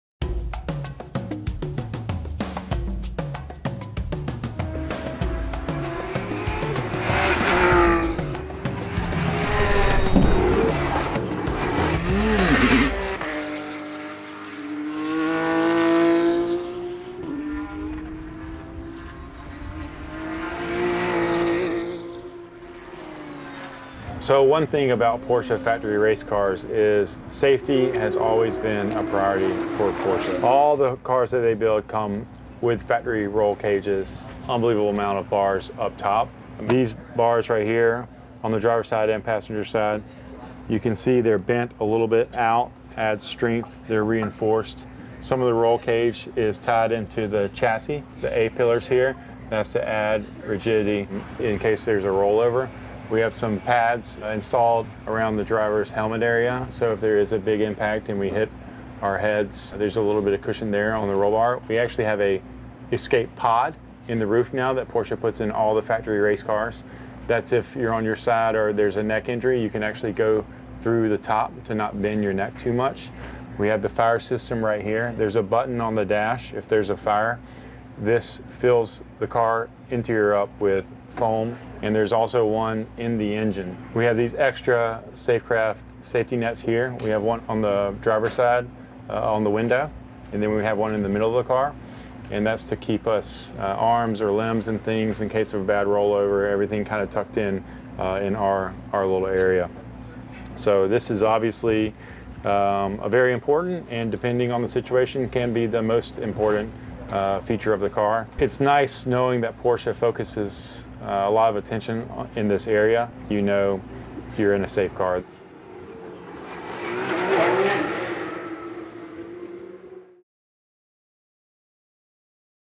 TECH TALKS: Porsche 992 GT3 Cup Safety Features
Driver of the #12 311RS Motorsport GT3 Cup, Leh Keen, talks about the safety features Porsche has built into the new 992 GT3 Cup.